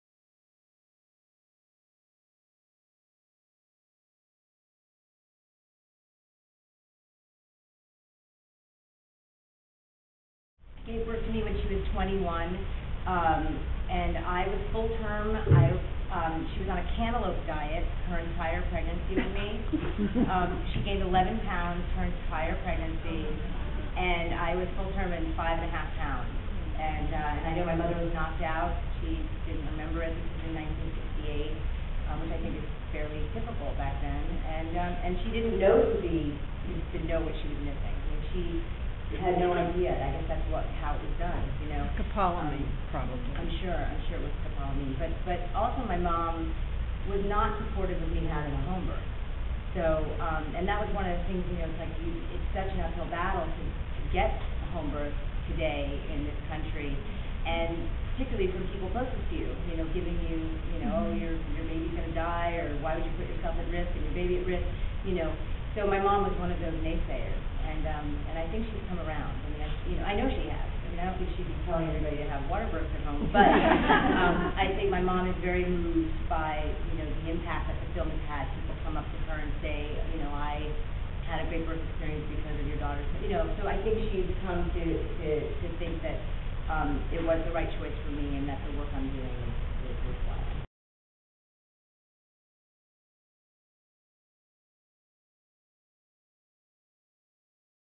Ricki Lake - Recipient of the CIMS 2010 Sharron S. Humenick Award
Ricki talks about when she was born and how her mother feels about the birth choices that Ricki made.

For more information about the Coalition for Improving Maternity Services (CIMS) and the Mother Friendly Childbirth Initiative,